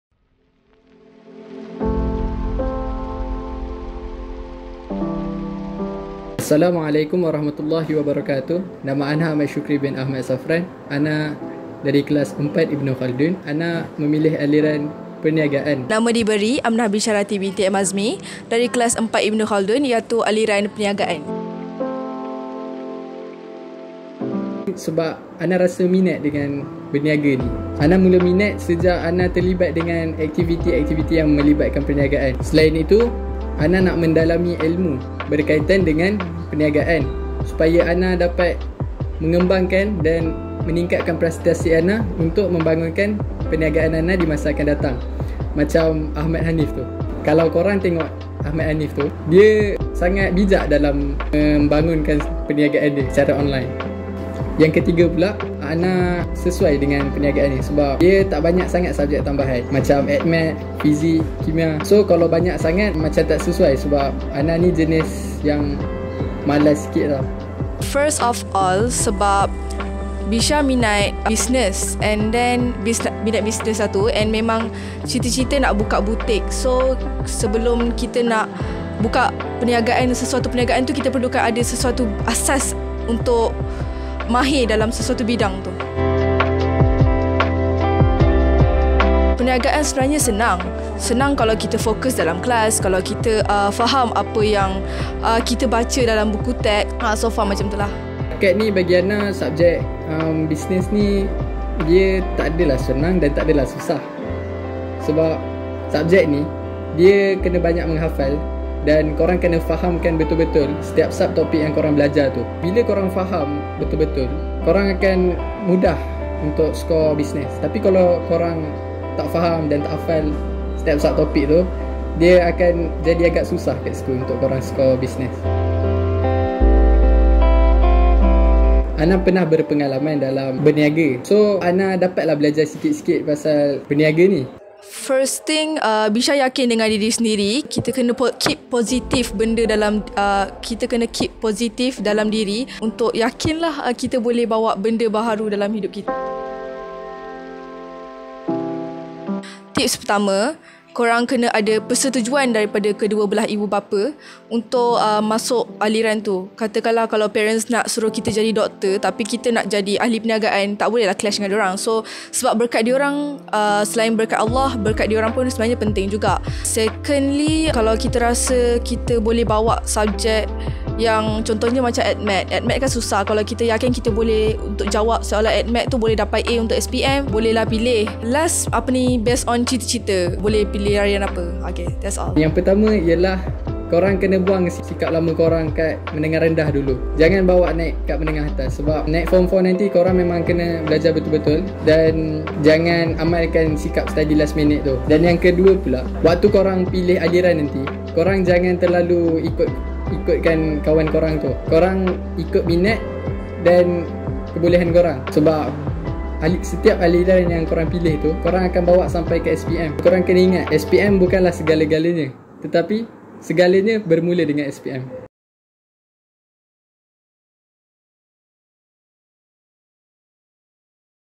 ALIRAN PERNIAGAAN | ALIRAN TINGKATAN 4
TEMU RAMAH PELAJAR PERNIAGAAN•
Aliran perniagaan adalah proses barang dalam edaran perubahan morfologi berlaku, iaitu bentuk kewangan ke dalam bentuk komoditi ke dalam proses bentuk wang, bersama-sama dengan berlakunya hubungan perdagangan, pemindahan hak milik barangan berlaku.
Adakah mata pelajaran yang dipelajari susah dan apa yang mebuatkan mereka yakin untuk membawa mata pelajaran baru?
Ayuh saksikan perkongsian daripada saudara Ahmad Shukri dan saudari Amnah Bisyarati daripada kelas 4 Ibnu Khaldun tentang aliran perniagaan di Youtube Alabang Production!